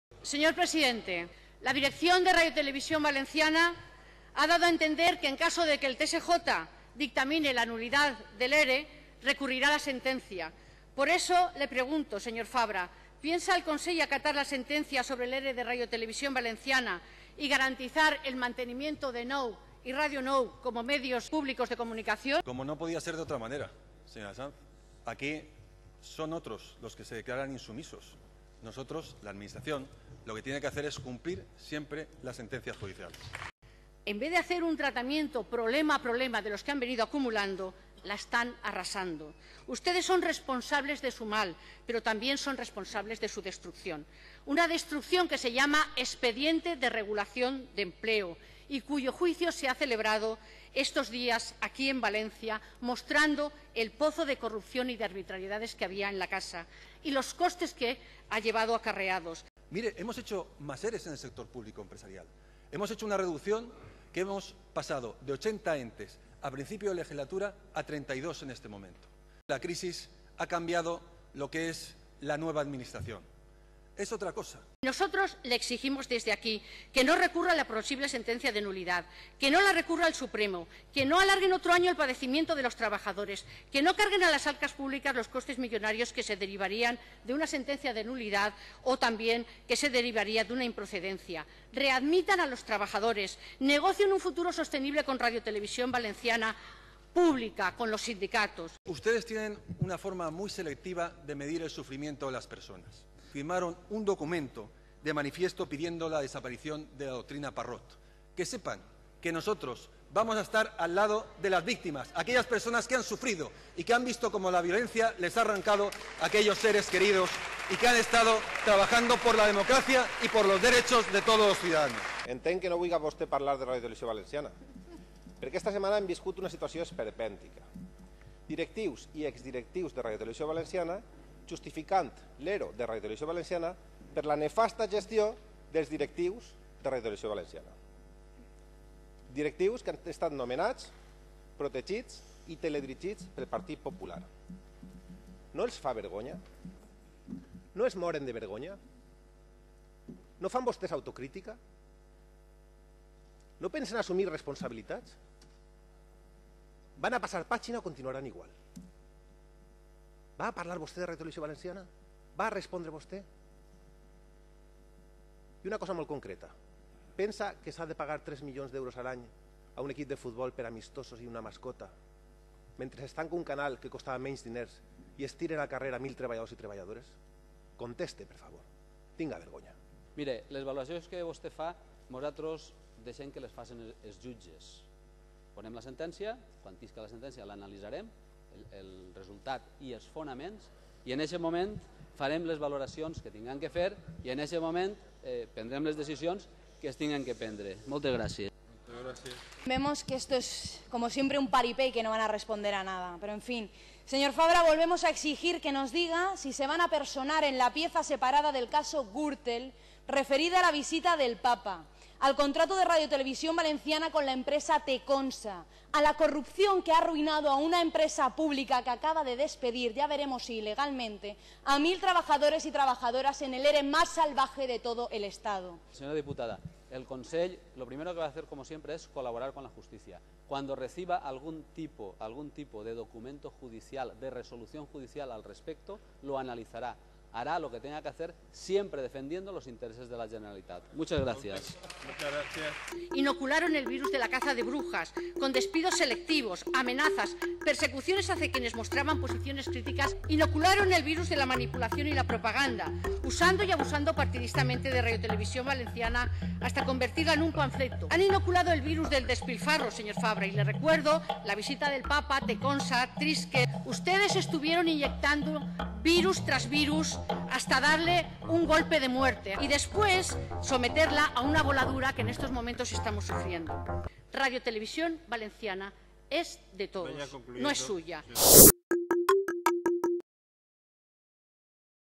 El Consell acataria la nul·litat de l'ERO de RTVV? La voladura controlada d'una televisió pública.
Pregunta de control al president del Consell per part d'Esquerra Unida durant el Ple del 24 d'octubre. Una setmana abans, es celebrava el judici sobre l'Expedient de Regulació d'Ocupació (ERO) de RTVV, que va deixar al carrer prop de 1.000 professionals malgrat una rectificació d'última hora que va repescar 186 treballadors (fonamentalment tècnics, per tal de garantir la continuïtat d'unes emissions mantingudes bàsicament amb reposicions). La Direcció de RTVV havia deixat entendre que, en cas que el TSJ dictamine la nul•litat de l'ERO, recorreria la sentència, allargant el procés més d'un any, amb el consegüent patiment per a les persones afectades i l'increment del cost per a les arques públiques. Mentrestant s'hi privatitzava la producció de bona part dels programes per a NOU.

La síndica del GP Esquerra Unida va preguntar al president Fabra: "Pensa el Consell acatar la sentència sobre l'ERO de RTVV i garantir el manteniment de NOU i Ràdio 9 com a mitjans públics de comunicació?"

Ple de Les Corts del 24 d'octubre de 2013.